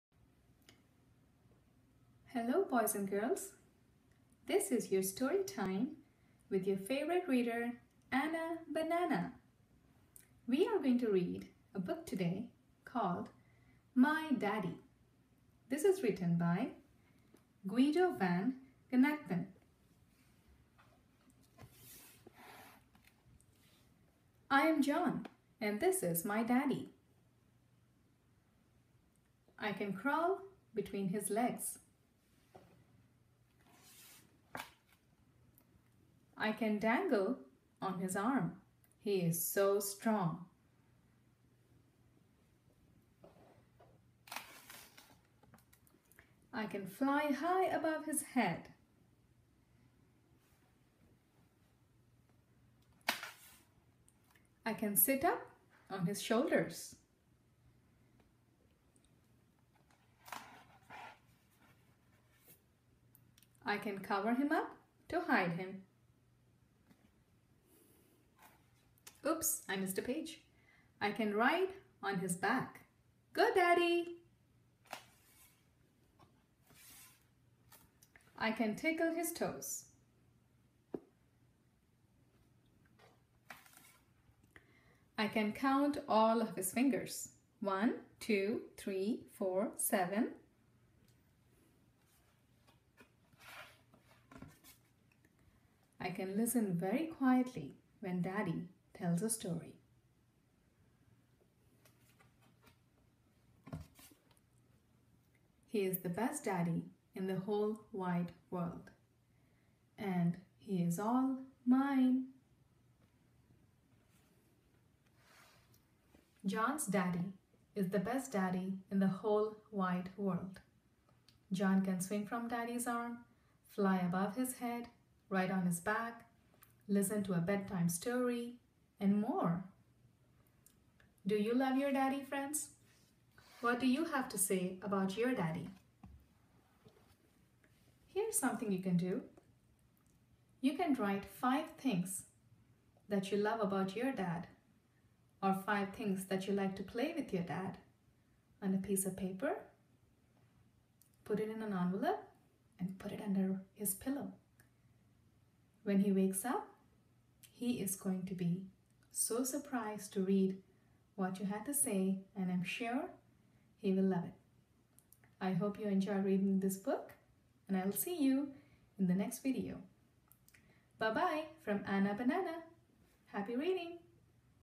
My Daddy - children's book read aloud
Guido Van Genechten
Story time for kids with Ana Banana